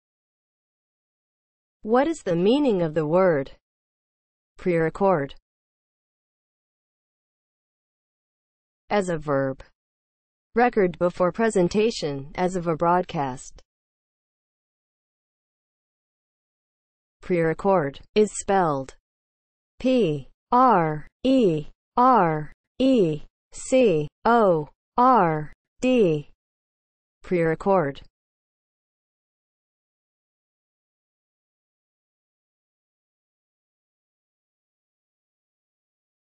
What is the meaning of the word PRERECORD?
Definition and spelling of the word PRERECORD.

(00:00) Intro
(00:07) As a verb
(00:15) Spelling

As a verb (00:07):
• record before presentation, as of a broadcast